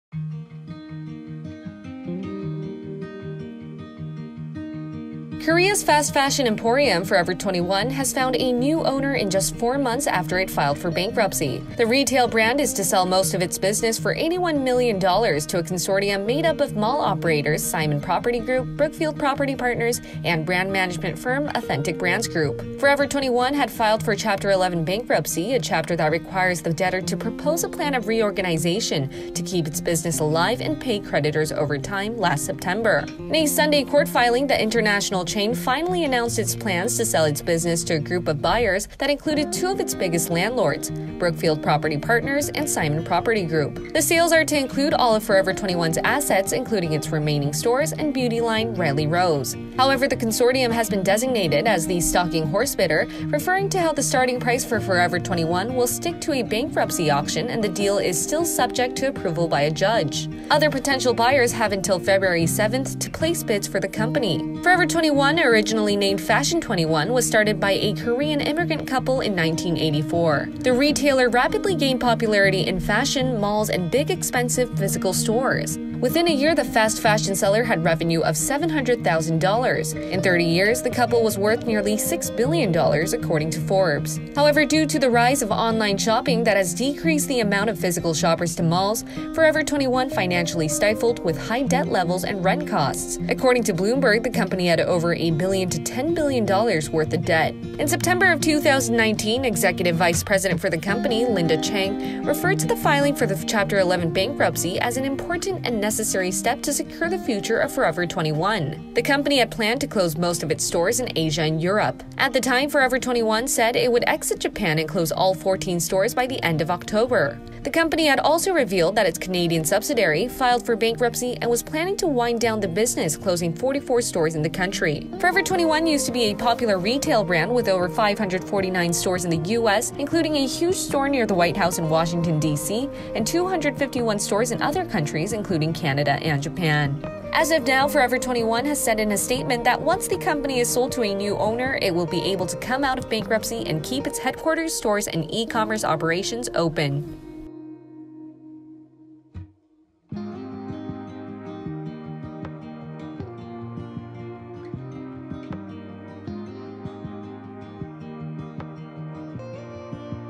Korean fast-fashion brand Forever 21 finds new owner in 4 months after filing for bankruptcy
Korea’s fast-fashion emporium, Forever 21 has found a new owner in just four months after it filed for bankruptcy. The retail brand is to sell most of its business for $81 million to a consortium made up of mall operators Simon Property Group, Brookfield Property Partners and brand management firm Authentic Brands Group. Watch this video, for more information.